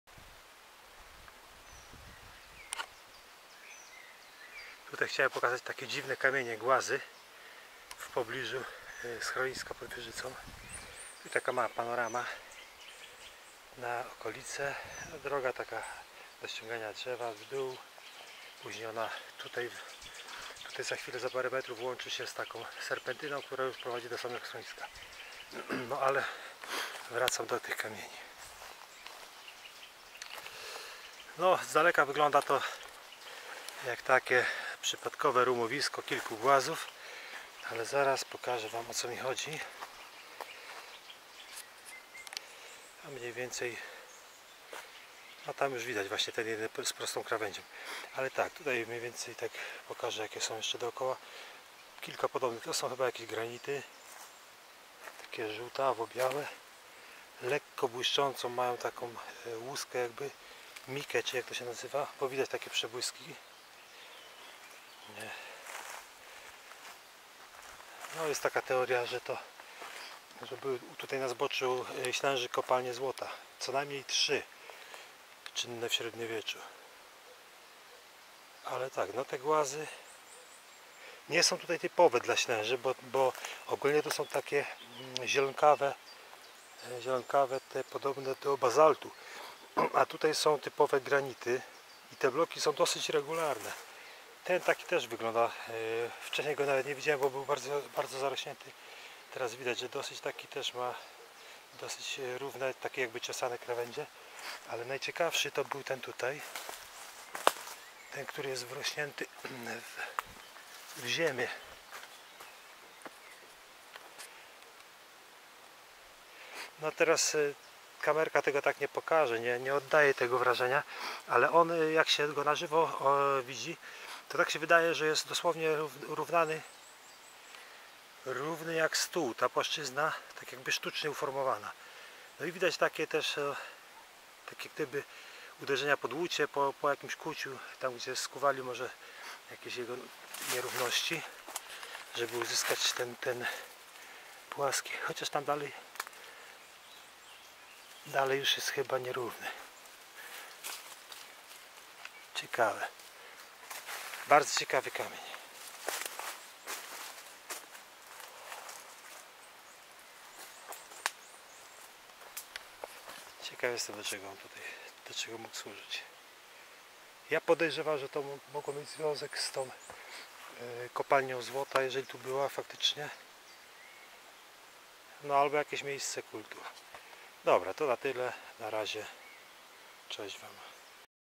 Tajemnicze głazy w Masywie Ślęża. Ruiny czy tylko przypadkowe kształty?
To miejsce już wcześniej chciałem pokazać, ale nie miałem przy sobie kamerki kiedy pierwszy raz tam trafiłem. Moim zdaniem nie jest to naturalne rumowisko skalne.